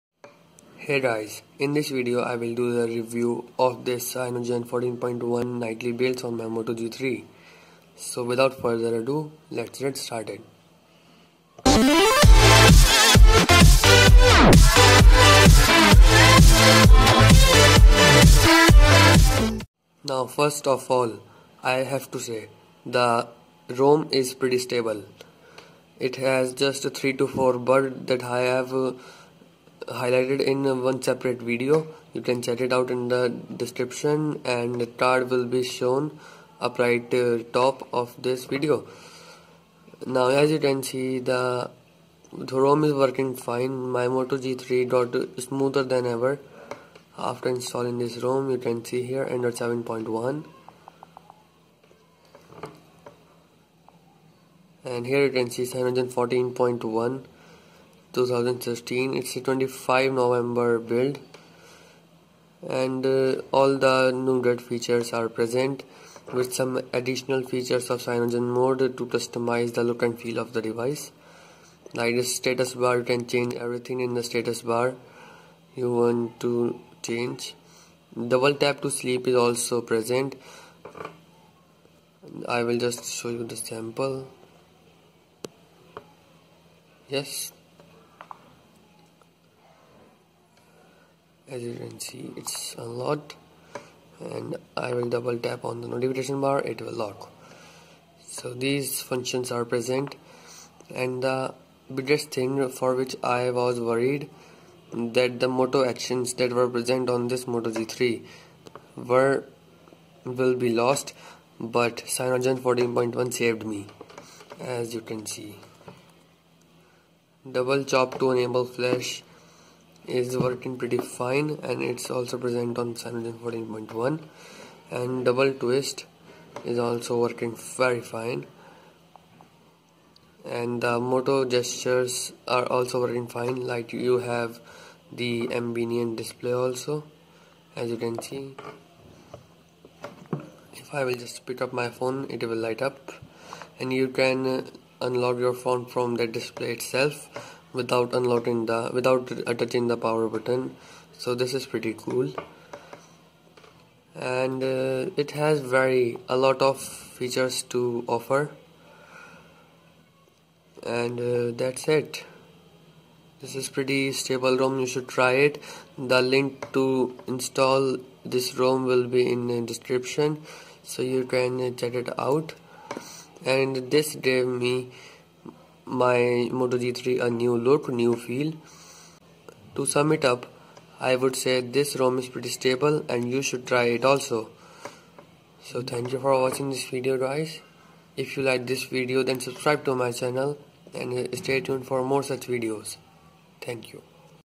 Official Cm 14.1 nightly on Moto G3 Review!
Hey guys! In this video I will do the full in depth review of the official Nightly builds of CM 14.1 based on Android Nougat 7.1 on Moto G 3rd gen (2015). So let's get started.